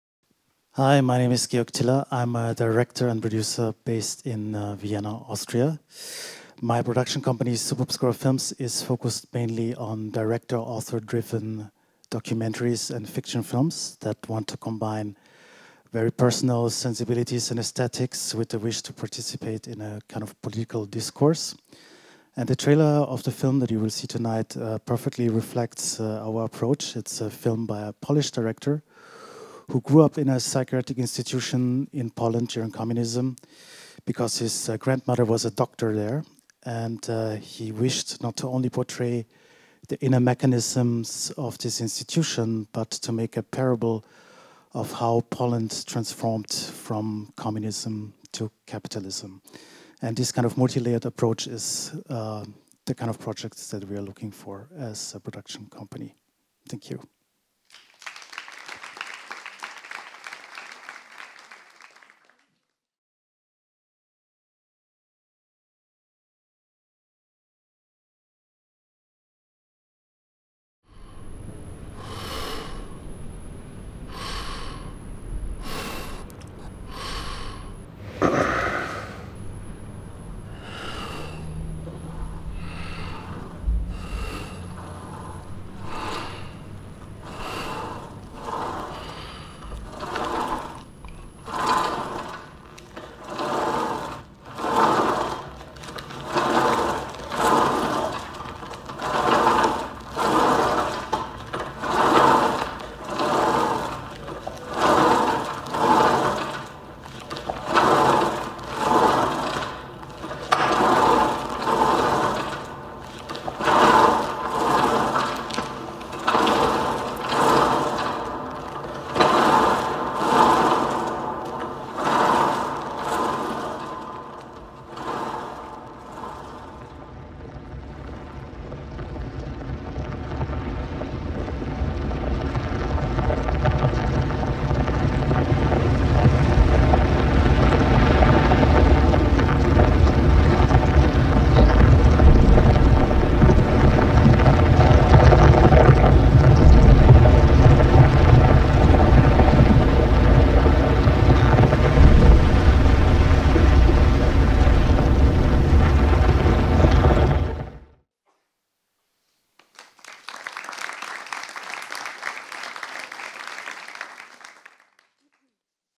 Emerging Producers 2019: Georg Tiller / Austria
This presentation took place during the 22nd Ji.hlava International Documentary Film Festival.

After unfinished philosophy and theatre studies, Georg Tiller (*1982, Austria) studied film and television with Harun Farocki, film directing and cinematography with Michael Haneke and Christian Berger, and film directing at the German Film and Television Academy Berlin. Tiller has produced and directed several feature films, documentaries and experimental films. He worked as a Senior Partner at the production company Mengamuk Films in Germany and is currently Managing Director of Subobscura Films in Austria.